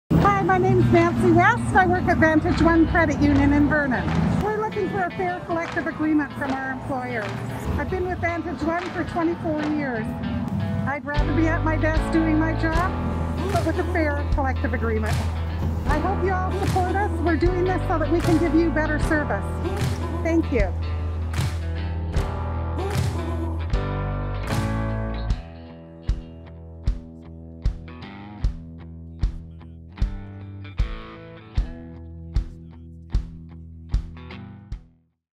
Nancy on the VantageOne picket line
MoveUP member Nancy tells us what her colleagues at VantageOne Credit Union are fighting for.